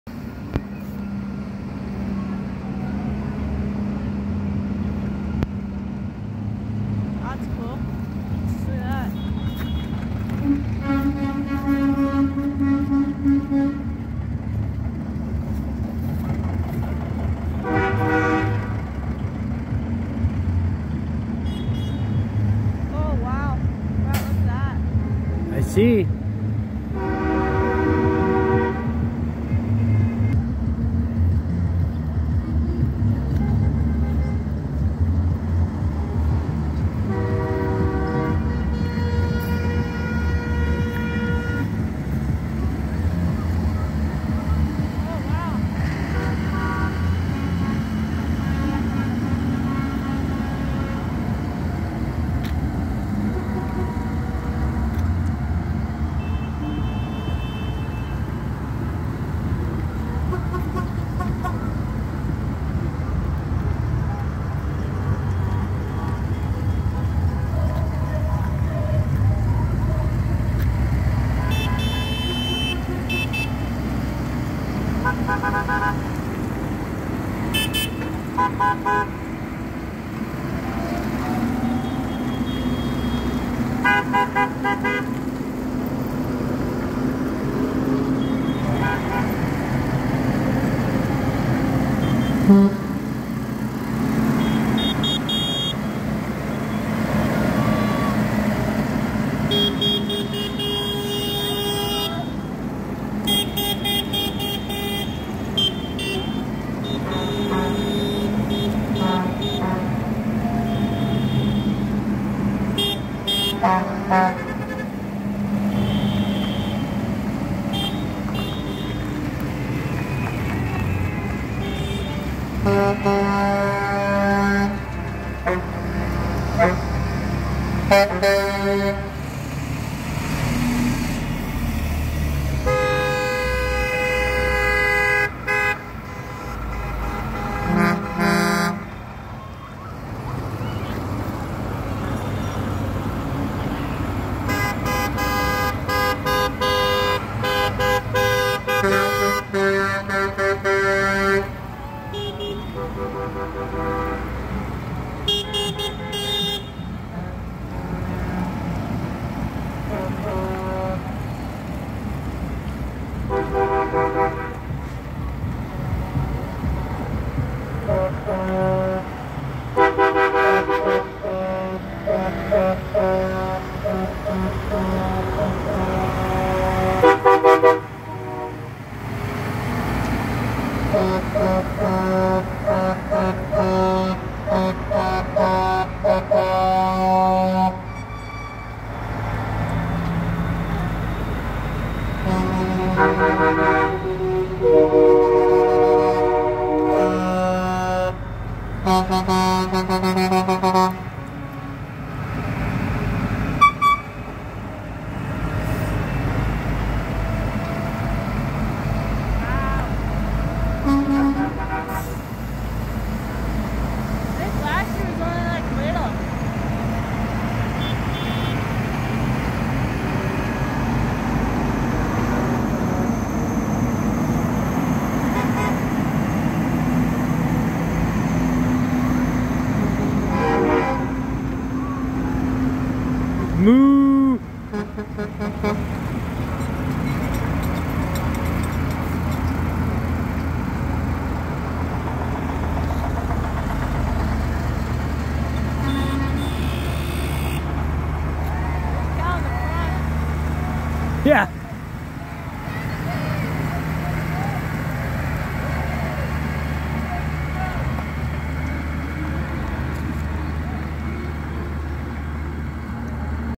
December 23, 2023
this is the 2023 Christmas tractor parade from armstrong bc Canada.